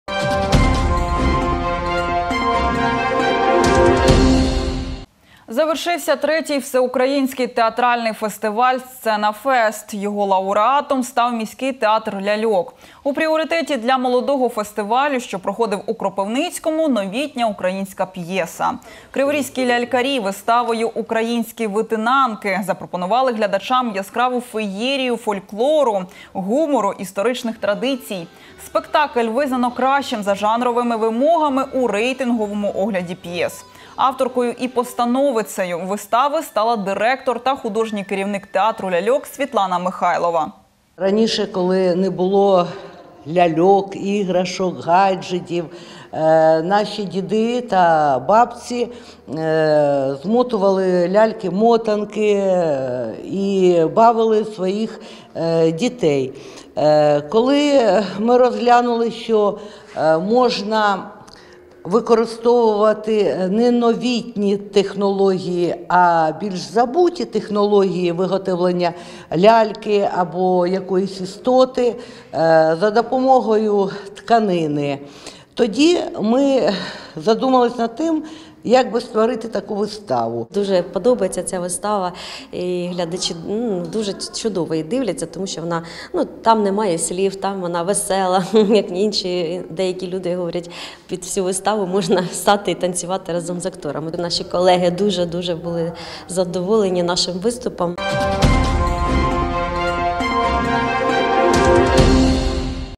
Черговий успіх: театр ляльок став лауреатом Всеукраїнського фестивалю
Завершився третій Всеукраїнський театральний фестиваль "Сцена-Фест". Його лауреатом став міський театр ляльок. У пріоритеті для молодого фестивалю, що проходив у Кропивницькому - новітня українська п'єса. Криворізькі лялькарі виставою "Українські витинанки" запропонували глядачам яскраву феєрію фольклору, гумору, історичних традицій. Спектакль визнано кращим за жанровими вимогами у рейтинговому огляді п'єс. Авторкою і постановницею вистави стала директор та художній керівник театру ляльок Світлана Михайлова.